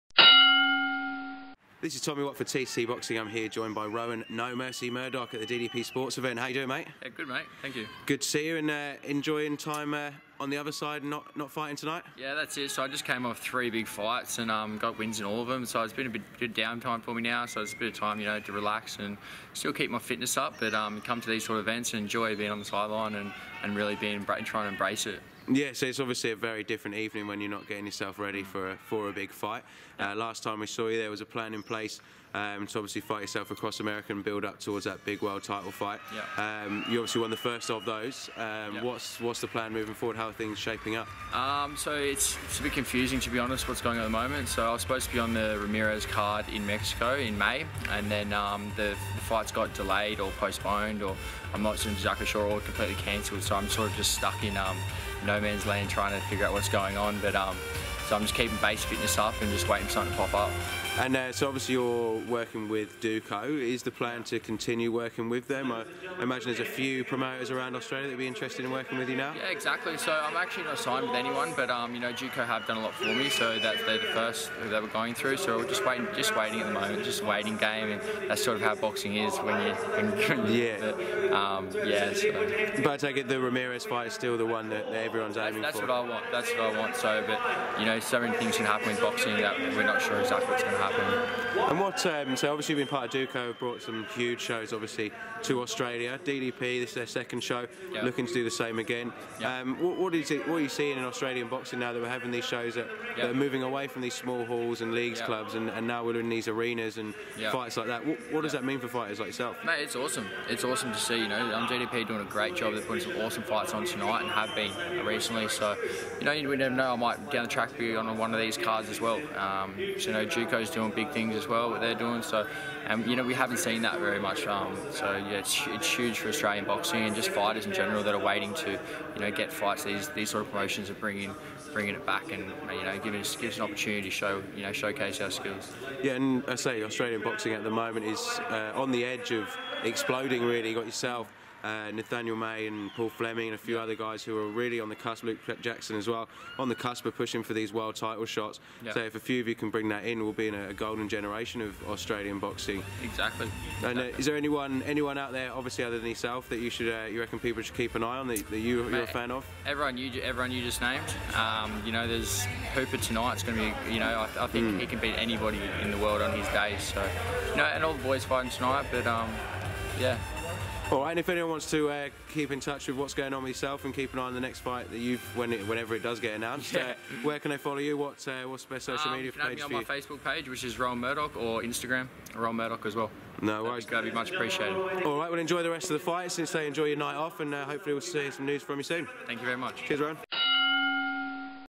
ROHAN 'NO MERCY' MURDOCH EDGING CLOSER TO RAMIREZ WORLD TITLE FIGHT
Australia's Rohan 'No Mercy' Murdoch discusses his plans for 2018 and hopes to face WBO Super Middleweight Champion Gilberto Ramirez (37-0) soon.